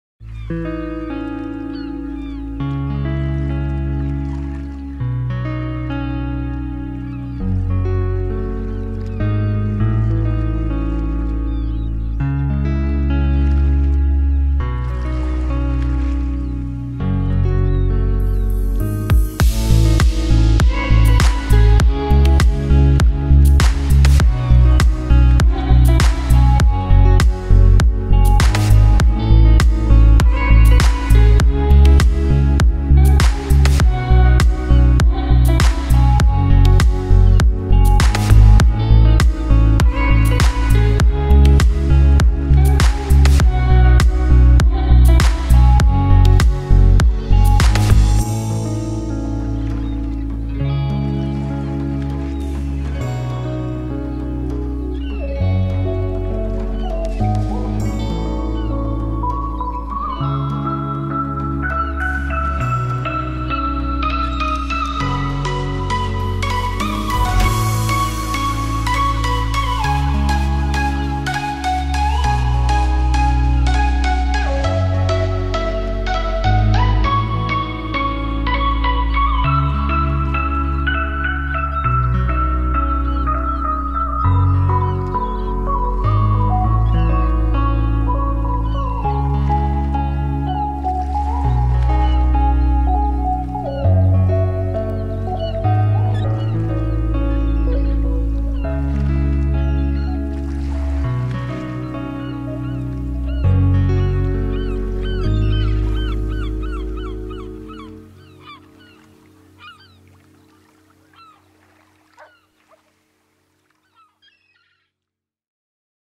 Fortress in the fjord, Drone 4K
"Munkholmen" is located in the fjord right outside Trondheim - Once a monastery in the 1100s it was rebuilt into a fortress by the danes in 1660. The castle fortress of "Steinvikholmen"was built for arch bishop Olav Engelbrektsson in 1524. In its prime this castle was considered the strongest fortification in Norway.

Filmed with a Mavic 2 Pro